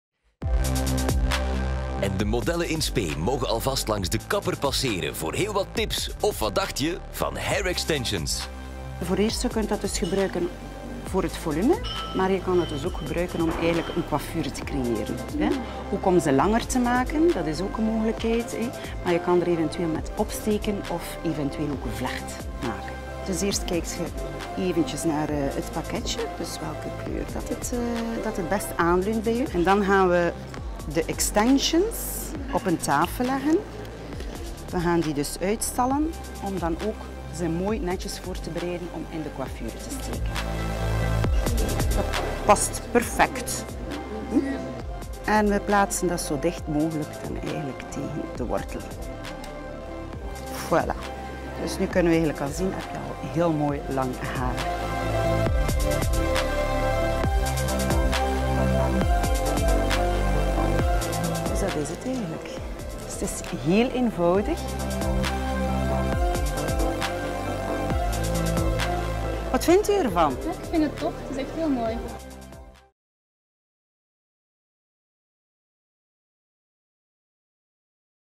Kruidvat - Franck Provost - extensions (2015)
Probeer de haar extensions van Franck Provost met professionele vezels van kapperskwaliteit, nu bij Kruidvat. Bekijk het resultaat!